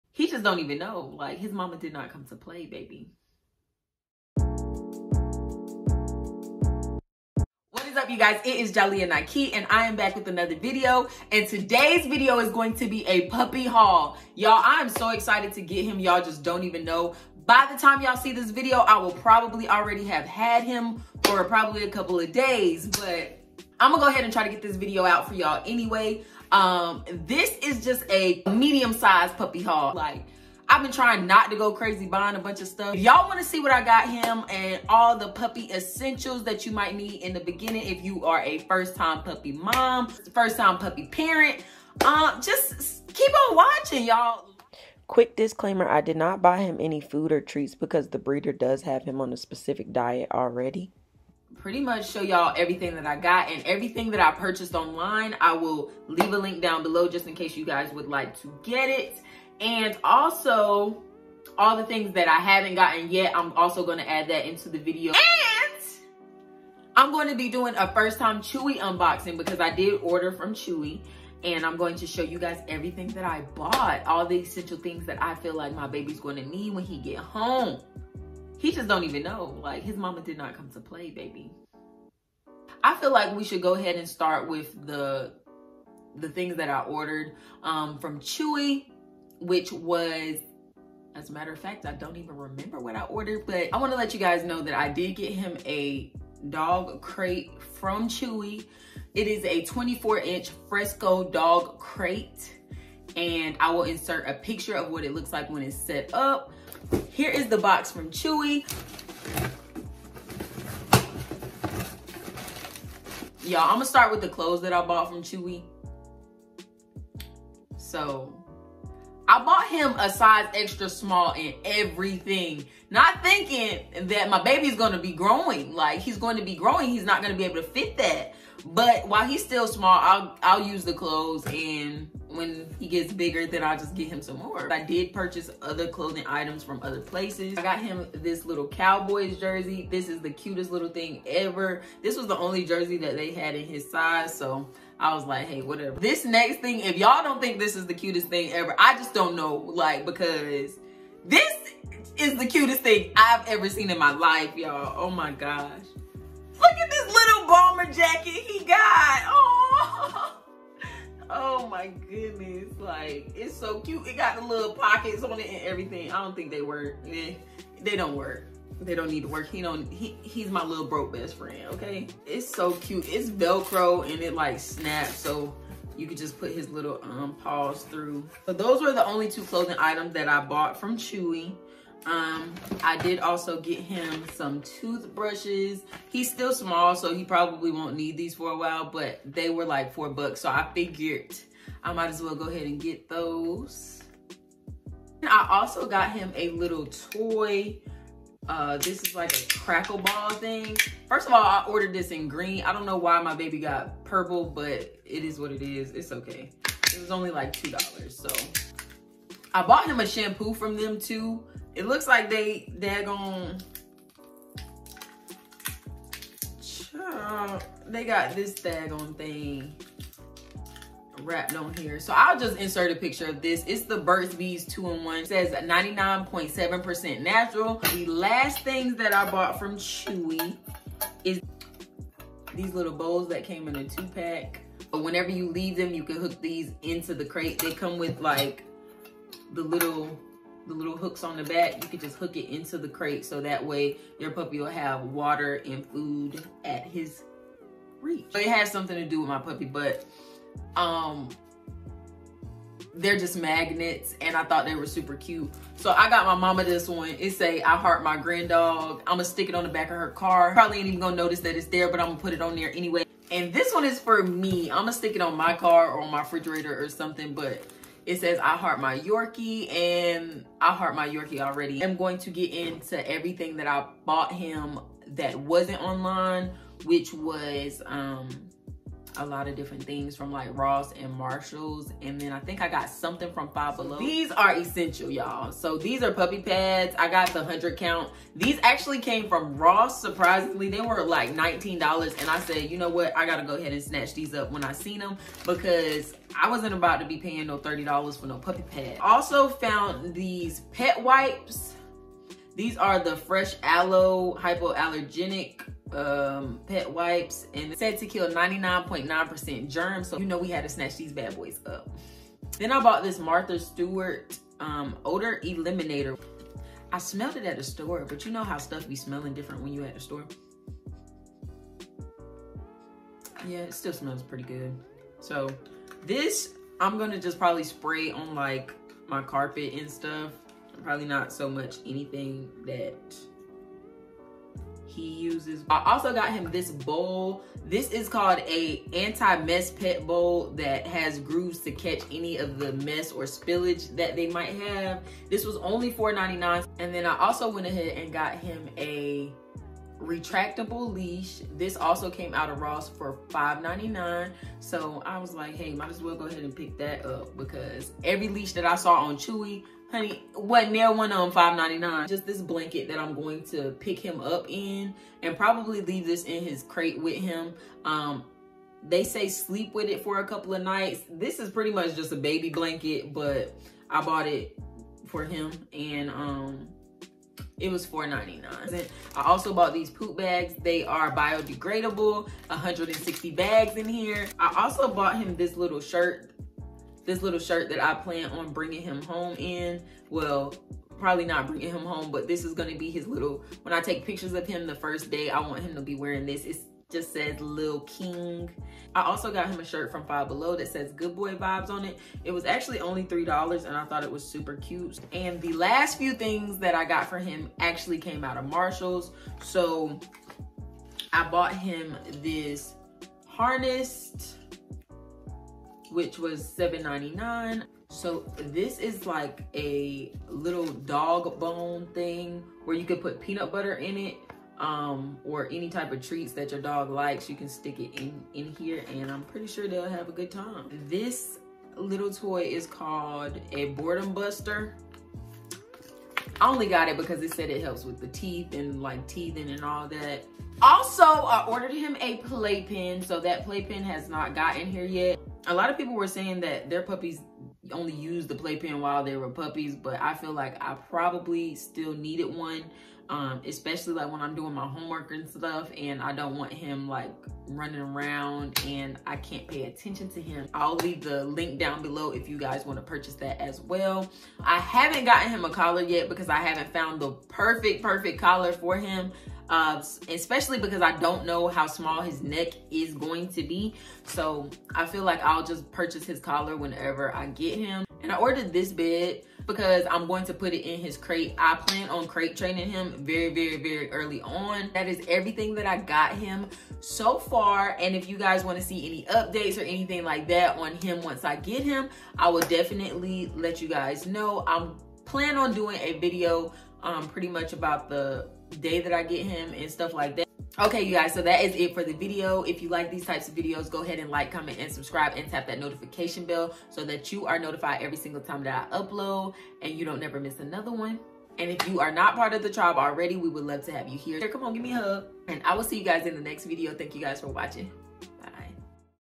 HUGE NEW PUPPY HAUL !!! YORKIE PUPPY ! FIRST TIME CHEWY UNBOXING!!!!! | Jalia Naki
Hey babes, today’s video is a Puppy haul containing  everything I purchased for my new Yorkie puppy ! Well almost everything and All the puppy essentials that you need if you’re also a first time puppy Mom/ Parent!

Everything that I purchased online will be linked below if you guys are interested in buying some of these essential items for your fur baby! If you are considering getting a fur baby or even if you already have one this video is one you don’t want to miss!

Get this video to 1k views and I will let you guys meet him!

Frisco Fold & Carry Single Door Collapsible Wire Dog Crate, 24 inch
Ruff 'n Ruffus Portable Foldable Pet Playpen + Free Carrying Case + Free Travel Bowl
New World Gray Dog Bed | Bolster Dog Bed Fits Metal Dog Crates | Machine Wash & Dry